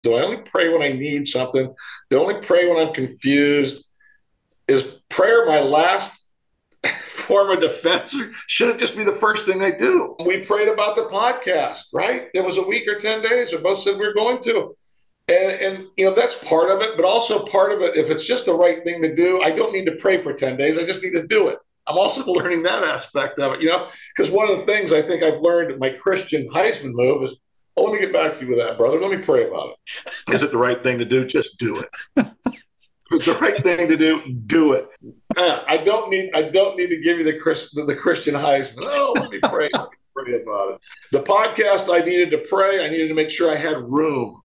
When do you pray? Last thing? Right away? Clint jokes about the "Christian Heisman". Do it!
Phil is a Missionary to Men, Author, and Emmy Award-Winning Journalist. We live on opposite coasts but share curiosity about timely cultural topics such as faith, family, sports (of course) and leadership. Join us. ClintHurdle.com.